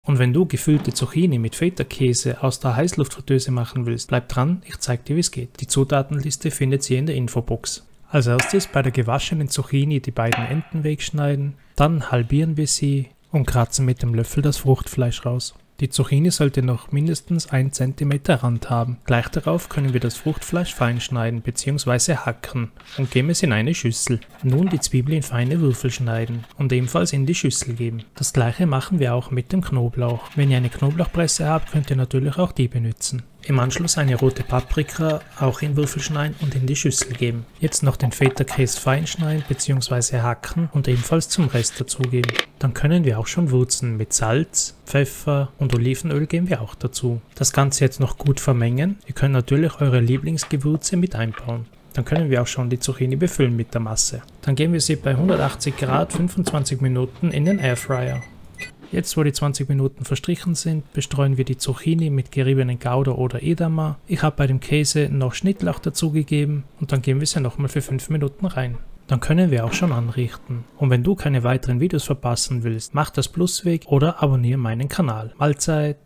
Gefüllte Zucchini mit Feta aus der Heißluftfritteuse – Einfach & Lecker!
In diesem Video zeige ich euch, wie ihr super leckere gefüllte Zucchini mit Feta Käse ganz einfach in der Heißluftfritteuse zubereiten könnt! 🌱🧀

Zutaten:
1 große Zucchini
100 g Fetakäse (z.B. Frischkäse, Kräuterfrischkäse)
1/2 Zwiebel, fein gehackt
1/2 Knoblauchzehe, fein gehackt
50 g geriebener Käse (z.B. Gouda oder Emmentaler)
1/2 rote Paprika, klein gewürfelt
1 EL Olivenöl
Salz und Pfeffer
Frische Kräuter (z.B. Petersilie, Schnittlauch), gehackt

Zubereitung:

Zucchini vorbereiten: Enden abschneiden, halbieren und das Fruchtfleisch herauskratzen. Es sollte ein Rand von ca. 1 cm bleiben.
Das Zucchinifleisch fein hacken und in eine Schüssel geben.
Zwiebel, Knoblauch und Paprika fein würfeln und ebenfalls in die Schüssel geben.
Fetakäse fein hacken und zur Mischung hinzufügen.
Mit Salz, Pfeffer und Olivenöl würzen – gerne auch eure Lieblingsgewürze dazu!
Die Zucchinihälften mit der Mischung füllen.
Bei 180 °C ca. 20-25 Minuten in die Heißluftfritteuse geben.
Nach 20 Minuten mit geriebenem Gouda oder Edamer bestreuen und für weitere 5 Minuten backen.
Anrichten und genießen!

In this video, I’ll show you how to make delicious stuffed zucchini with feta cheese, all done in the air fryer! 🌱🧀

Ingredients:

1 large zucchini
100 g feta cheese, finely chopped
1/2 onion, finely diced
1/2 garlic clove, minced (or use a garlic press)
1/2 red bell pepper, finely diced
1 tbsp olive oil
Salt and pepper to taste
Fresh herbs (e.g., parsley, chives), chopped (optional)
50 g shredded Gouda or Edam cheese (for topping)
Fresh chives, chopped (optional, for topping)
Instructions:

Prepare the Zucchini:
Wash the zucchini, cut off the ends, and slice it lengthwise. Use a spoon to scoop out the flesh, leaving about 1 cm of the zucchini shell.

Prepare the Filling:
Finely chop the scooped-out zucchini flesh and place it in a bowl.
Dice the onion and add it to the bowl.
Mince the garlic (or use a garlic press) and add it to the bowl.
Dice the red bell pepper and add it to the bowl.
Finely chop the feta cheese and mix it into the bowl.

Season the Filling:
Season the mixture with salt, pepper, and olive oil. Add any additional herbs or spices to taste. Mix well.

Stuff the Zucchini:
Fill the zucchini halves with the prepared mixture.

Cook in the Air Fryer:
Place the stuffed zucchini in the air fryer basket and cook at 180°C (356°F) for about 20-25 minutes.

Add Cheese Topping:
After 20 minutes, sprinkle shredded Gouda or Edam cheese on top of the zucchini. Optionally, add some chopped fresh chives. Return the zucchini to the air fryer and cook for an additional 5 minutes, until the cheese is melted and golden.

Serve and Enjoy:
Remove the stuffed zucchini from the air fryer, let it cool slightly, and serve.

Auf dem neusten Stand seid ihr auf meinen Sozialmedias!